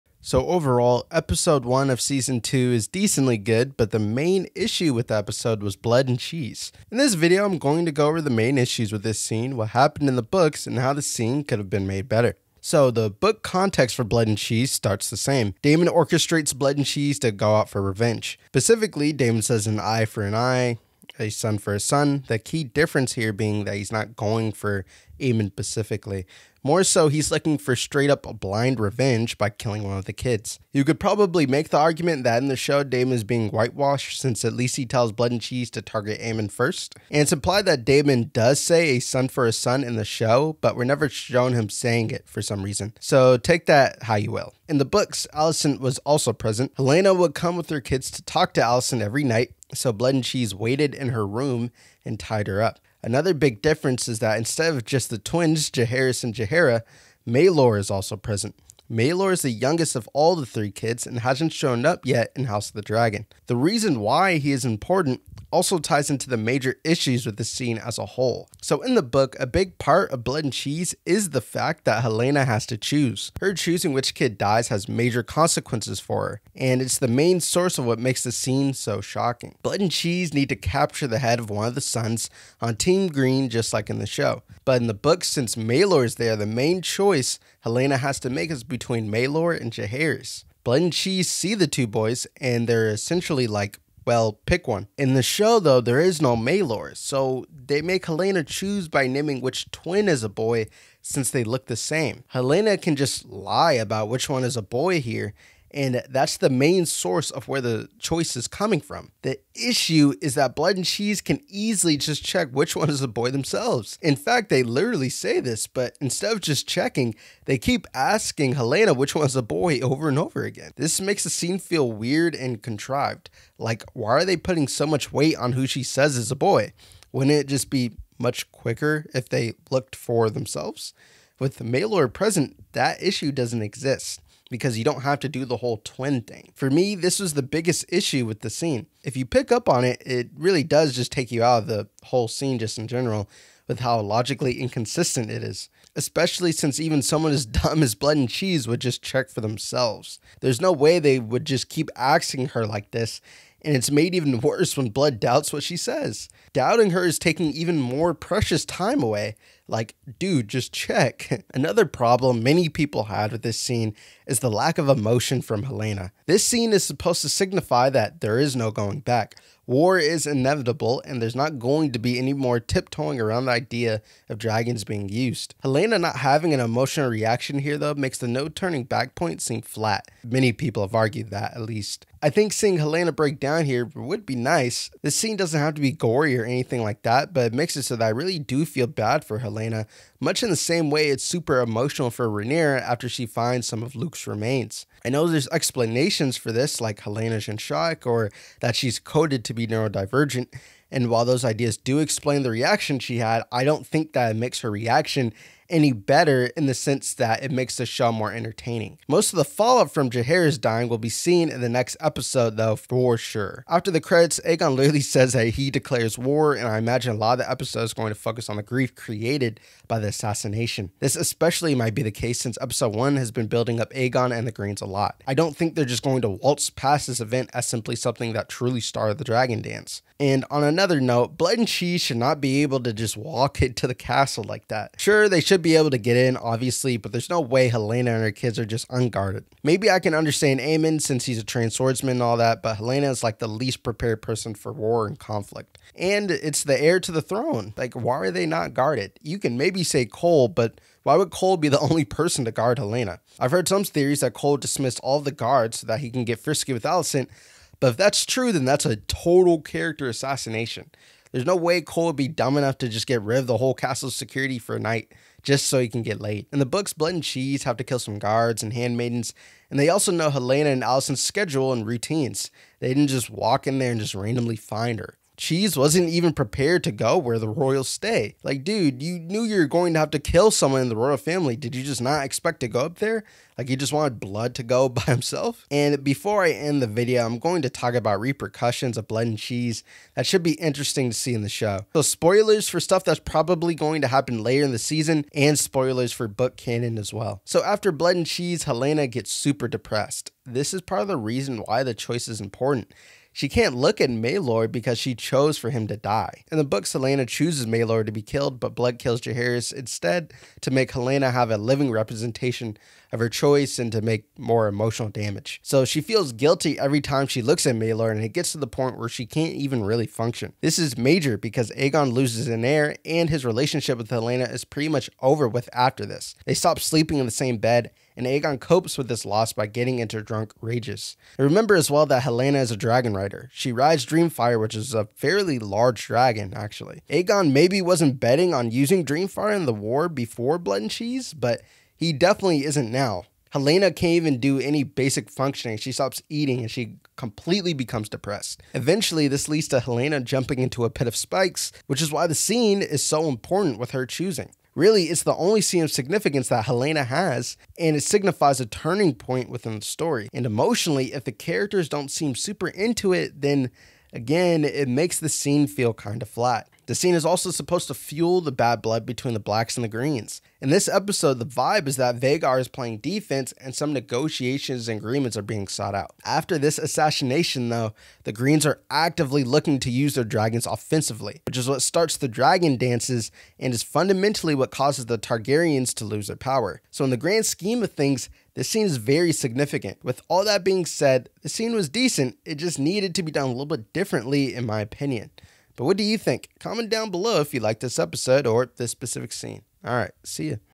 Blood and Cheese is not done well... House of the Dragon season 2 episode 1
House of the dragon is inspired by the book Fire and blood. to buy and support me